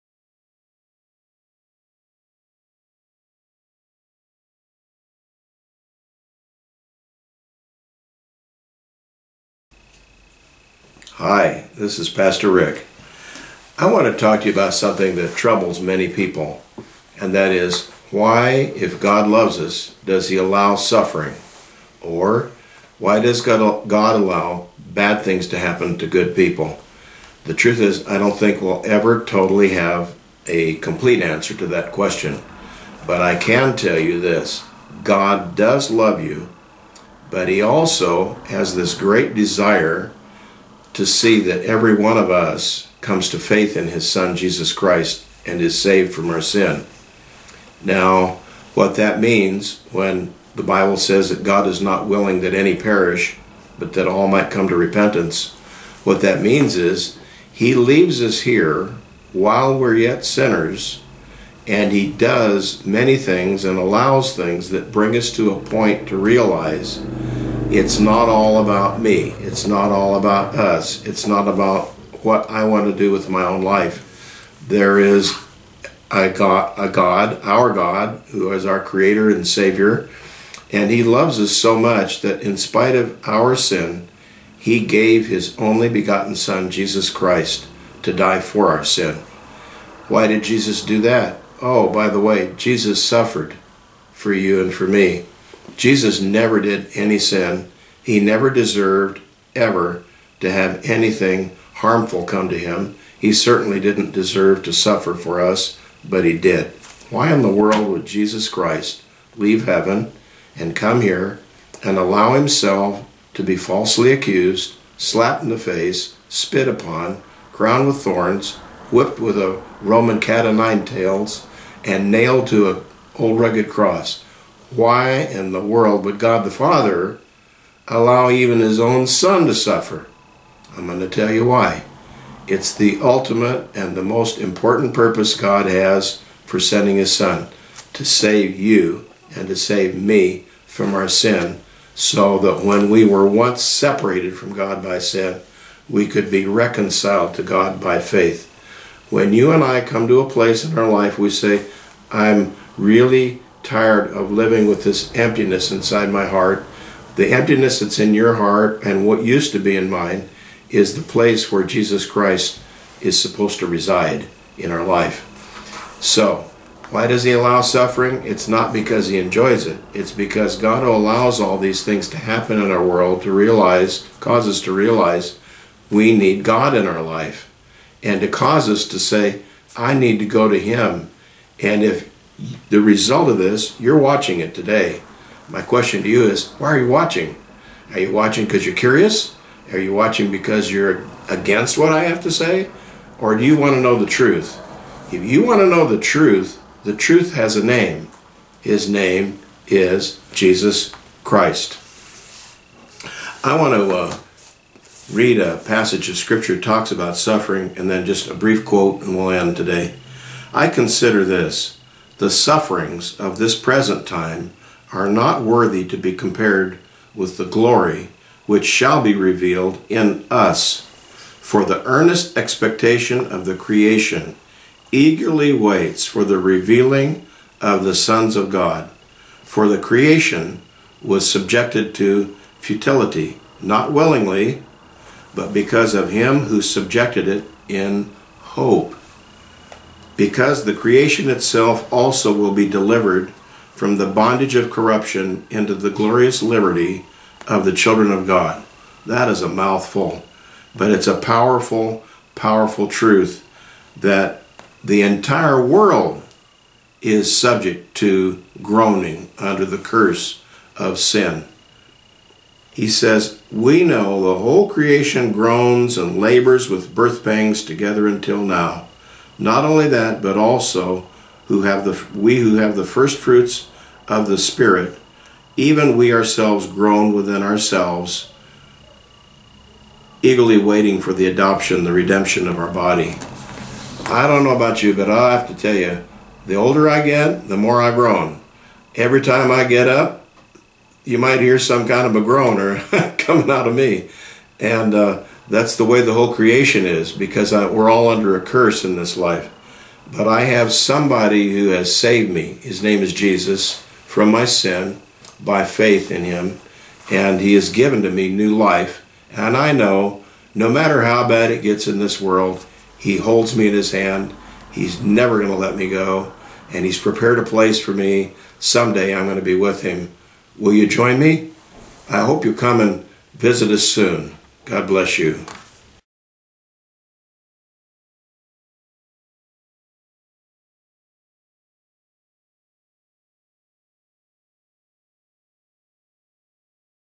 "Why Bad Things Happen to Good People" Living Hope w/ Pastor Rick Patterson week of March 16, 2015
"Why Does God let Bad Things Happen to Good People" Living Hope message with Pastor Rick Patterson week of March 16, 2015 episode 75

433 S. San Jacinto

1068 E. Mayberry